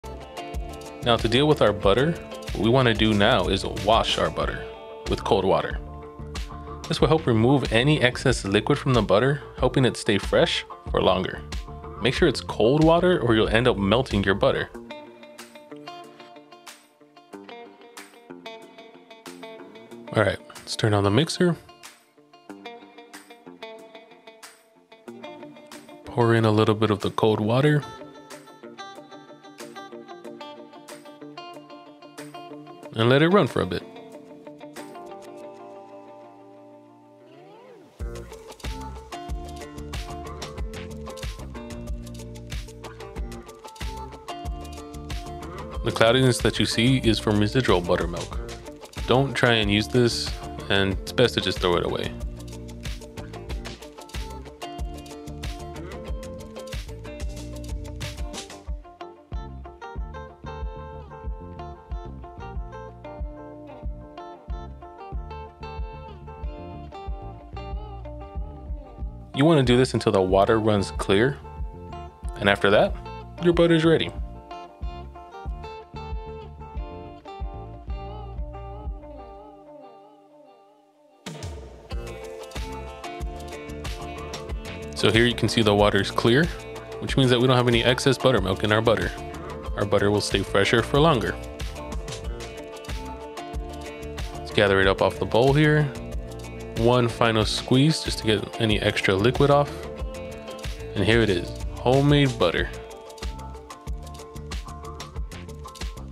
EASY HOMEMADE BUTTER
Join us at our main channel!
Homemade butter can be easily made by anyone! An important step in the process is to wash your butter. Watch this video to see why you should wash your butter.